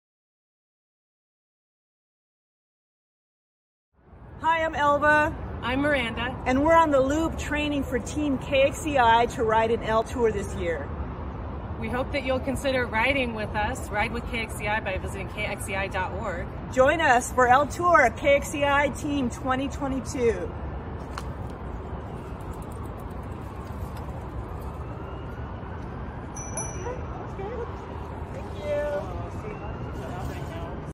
Ride with KXCI in the 39th Annual El Tour de Tucson
KXCI is putting together a team of people to Ride with KXCI in the 39th Annual El Tour de Tucson. You can help KXCI by joining our team. You’ll have lots of fun doing it, and you’ll make a big impact on KXCI. Choose the length that's right for you, or simply help support your favorite rider. When you Ride With KXCI, you’ll help bring more music and community to your friends and neighbors. Learn more on the Ride With KXCI page at KXCI.ORG.